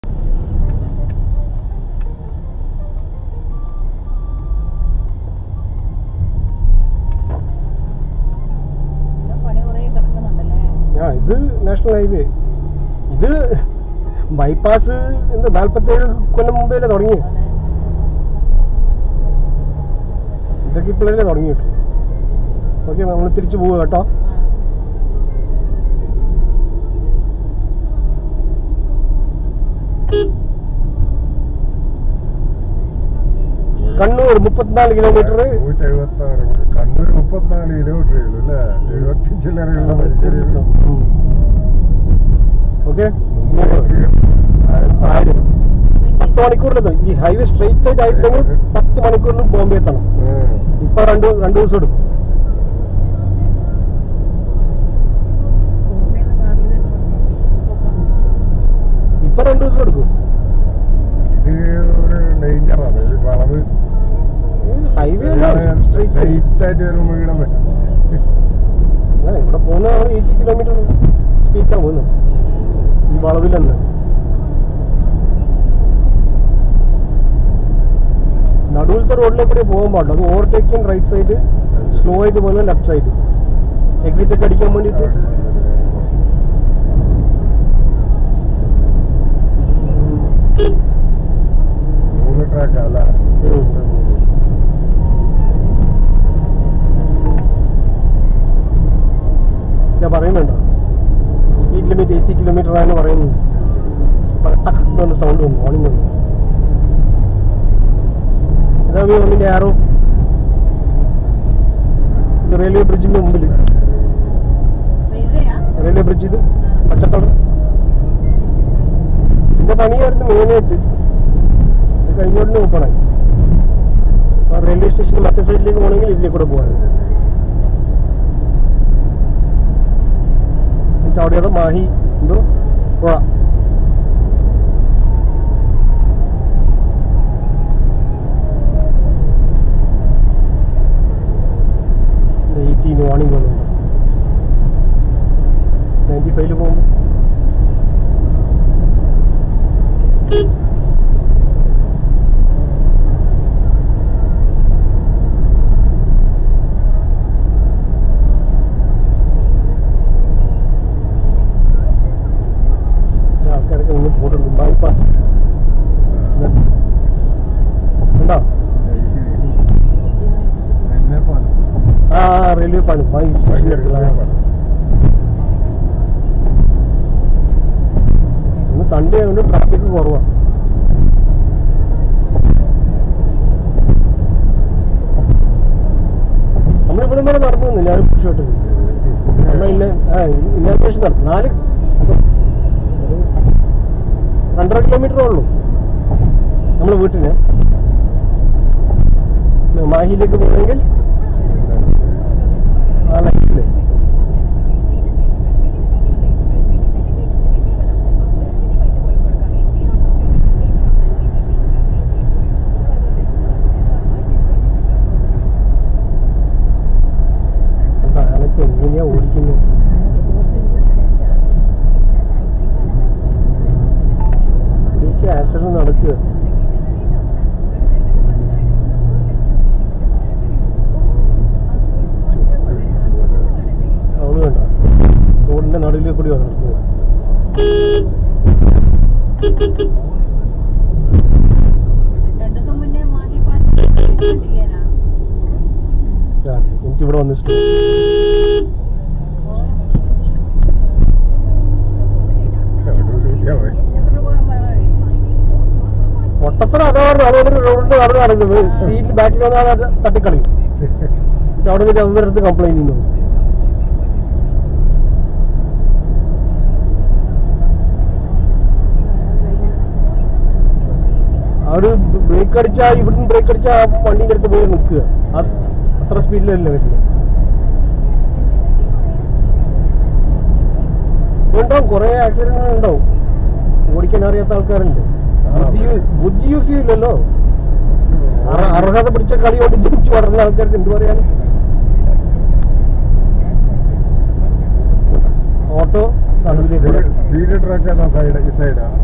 VID0006_Mar_10_2024_Sunday morning TRIP NH66 Mahe-Tellicherry By Pass trip
Mar_10_2024_Sunday morning TRIP NH66 Mahe-Tellicherry By Pass trip.
Tomorrow Mar_11_2024 Monday is going to be the official Inauguration of the Mahe - Tellicherry By-Pass to the Nation, the World  by the Prime Minister of India Mr Narender Modi .